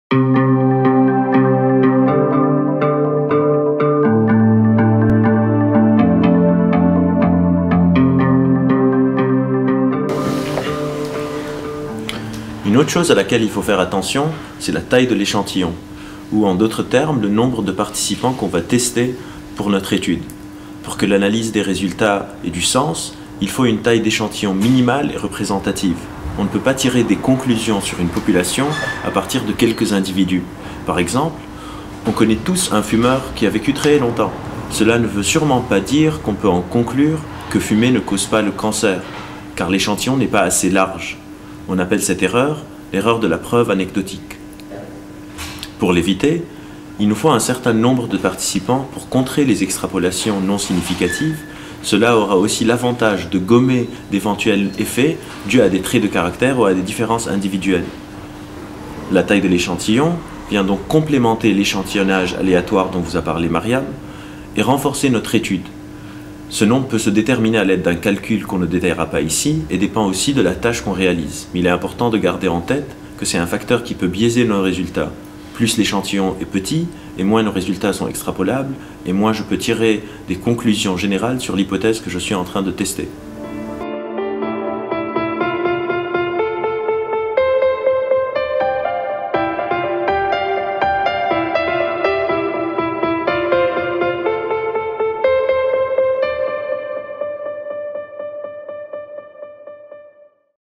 MOOC - Neurosciences - L'erreur de la preuve anecdotique MCAM-3/9
Maryam Chammat et Albert Moukheiber